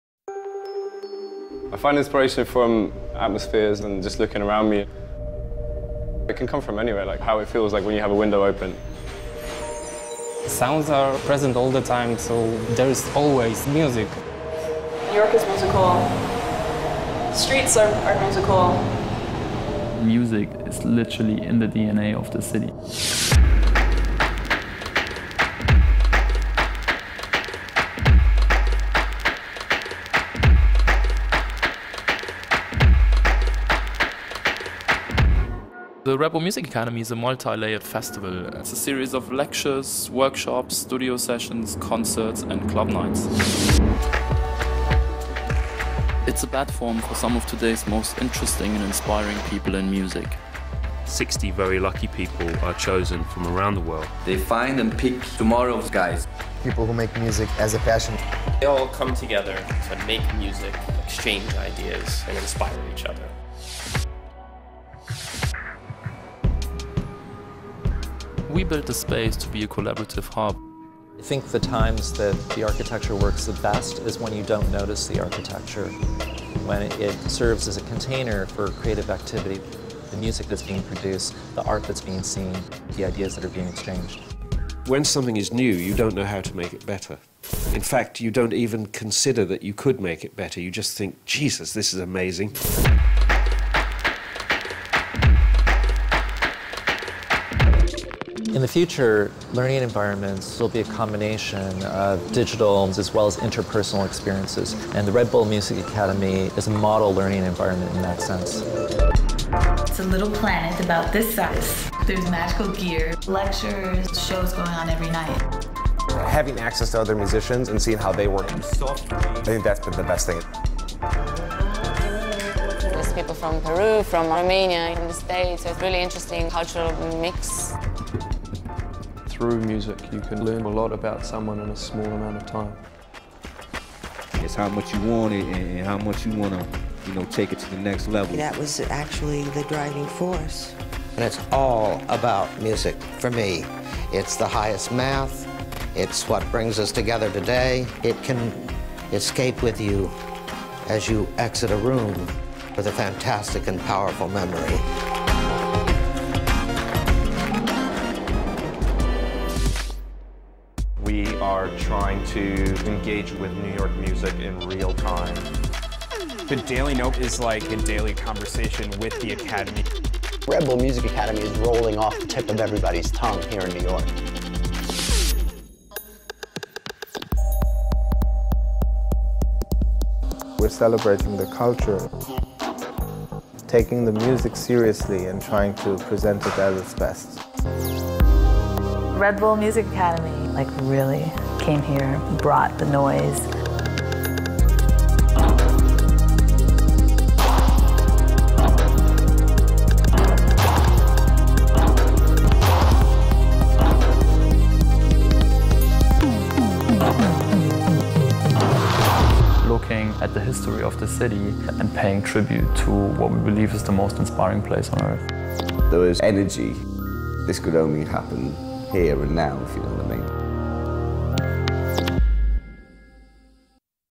Create, Collaborate, Celebrate - The Red Bull Music Academy 2013 in New York
A short clip about the Red Bull Music Academy 2013 in New York. 321 Artists, 34 Nights, 8000 Anthems, 1 City.

Find out more about the Red Bull Music Academy and see all New York lectures here: